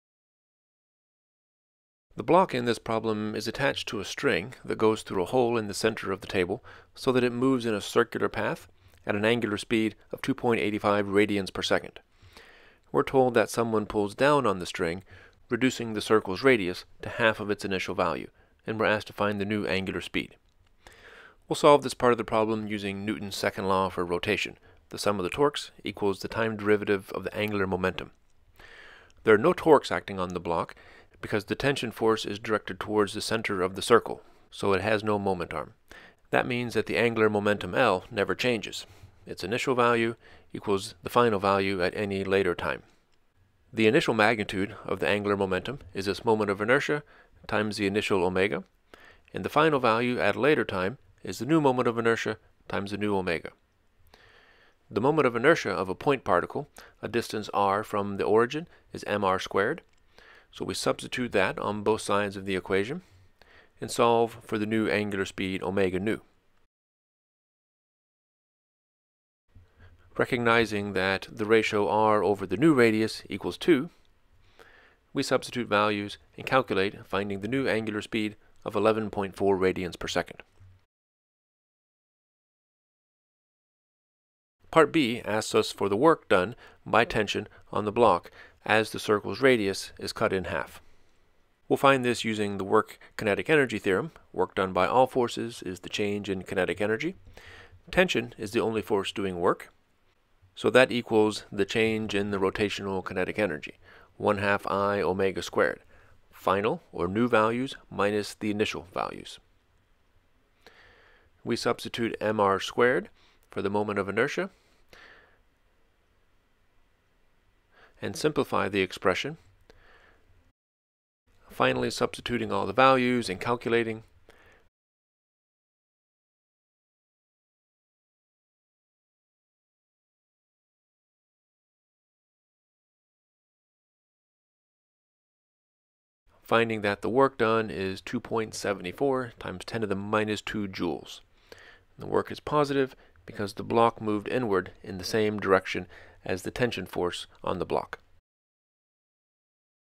Rotational Motion: Angular Momentum. Level 2, Example 1
A small, 0.0250-kg block is attached to a cord passing through a hole in the center of a frictionless table, as in the figure. The block moves with speed v along a circular path so that it revolves at an angular speed of 2.85 rad/s a distance of 0.300 m from the hole. The cord is then pulled from below, shortening the distance from the hole to 0.150 m. Find (a) the block’s new angular speed and (b) the work done by the tension force on the block.